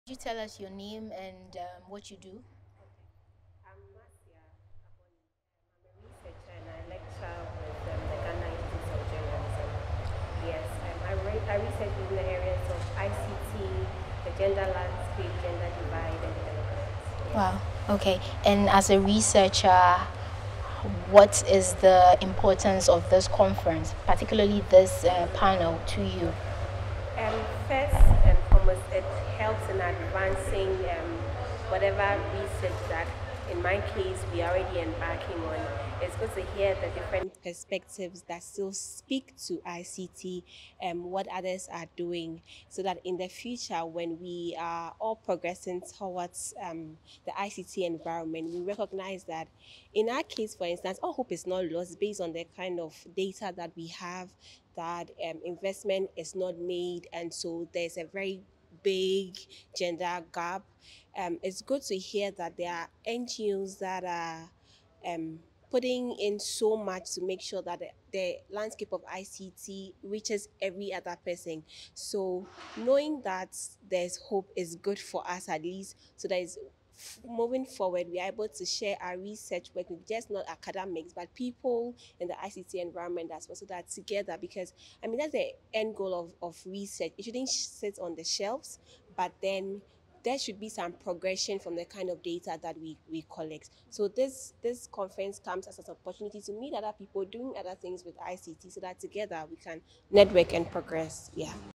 Ghana Tech Futures- Marcia Abonie panelist- Ghana Studies Conference 2022
Interview with panelist Marcia Atsufui Afi Abonie (Ghana Institute of Journalism). Abonie holds an MPhil Communication Studies. She is a member of the International Association of Media and Communication Research (IAMCR), and has a forthcoming chapter with Sey, A., Ansu-Kyeremeh, K.. titled "Chores, Mores and Digital Doors." in the book, Under the Radar. Routledge.

Videography: BEYOURSELF multimedia (Ibrahim), Tamale, Ghana

Interviewer: Fauziyatu Moro